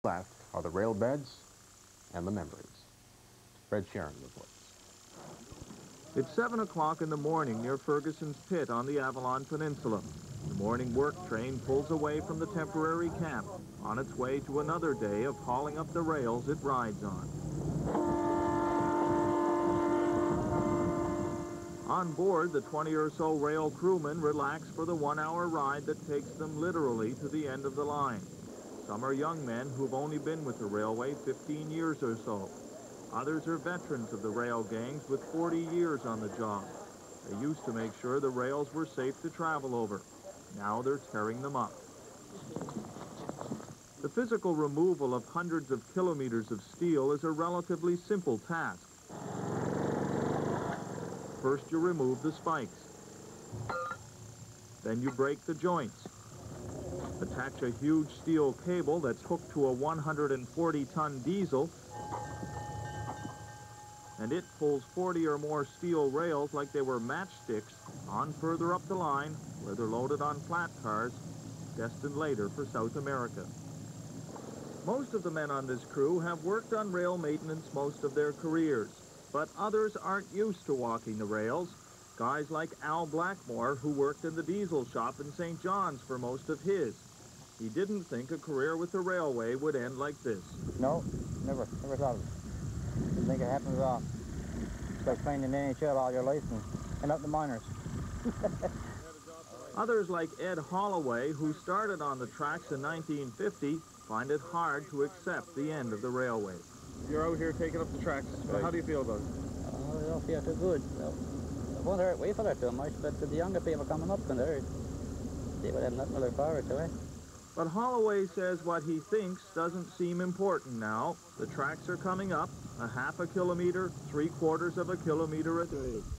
Newfoundland Railway #8 Track Removal near Ferguson's Pit
Downloaded from VHS CBC newscast. I take no credit for it.